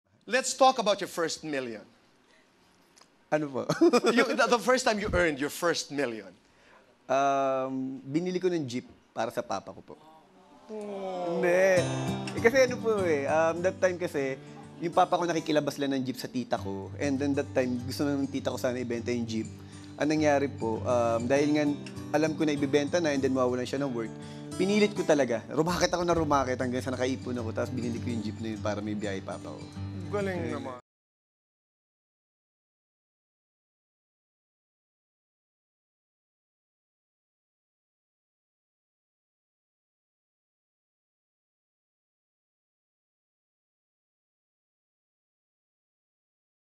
Coco shares what he did with his first million
Coco Martin reveals what he did with his first earnings.